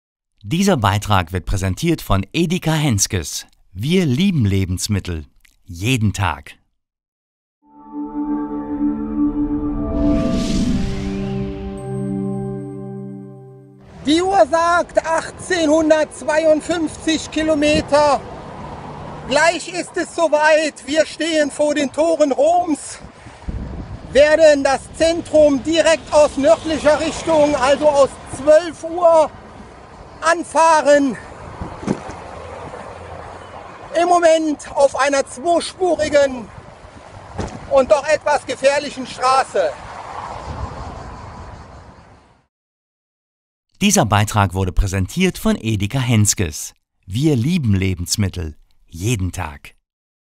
myREGIO.TV - AKTUELL: Sondermeldung
Es ist geschafft. Nach 19 nassen, heissen und anstrengenden Tagen
fahren unsere 3 Wassenberger in Rom ein.
Alles zu den Etappen 15 und 16 ab heute Abend, 18:00 Uhr.
Die letzte Etappe und die Ankunft in Rom ab morgen auf myREGIO.TV.